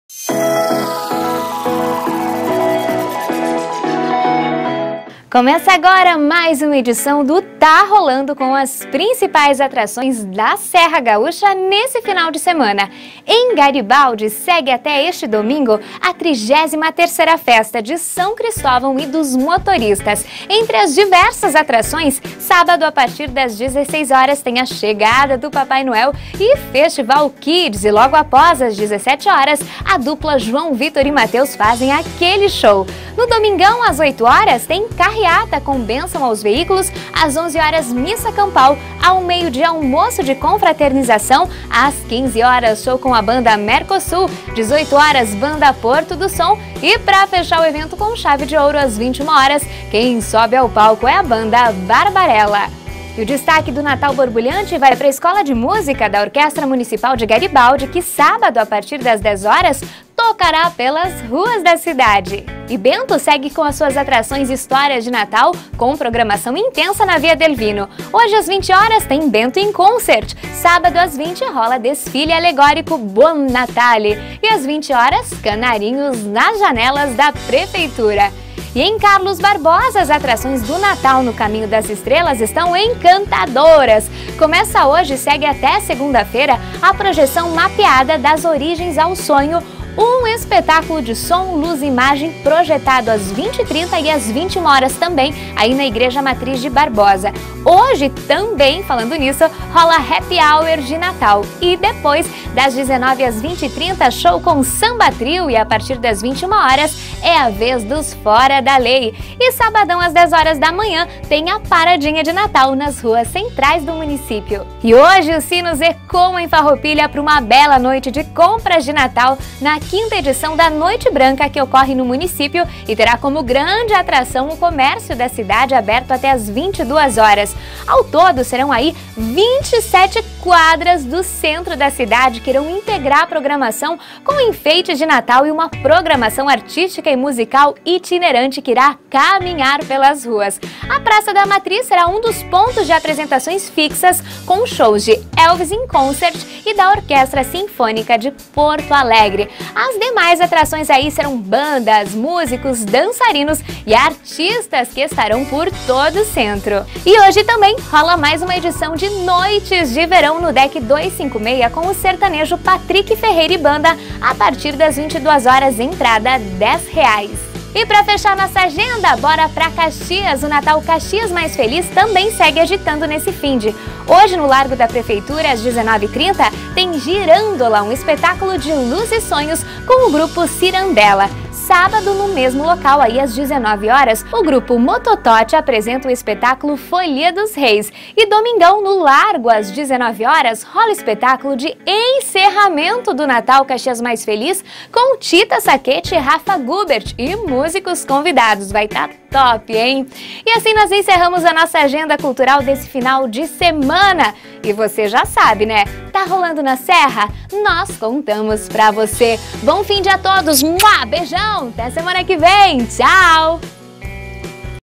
Portal Leouve - Tá Rolando
Confira a agenda cultural da Serra Gaúcha  desta sexta-feira (20), sábado (21) e domingo (22).

Apresentação: Lilian Donadelli/ Grupo RSCOM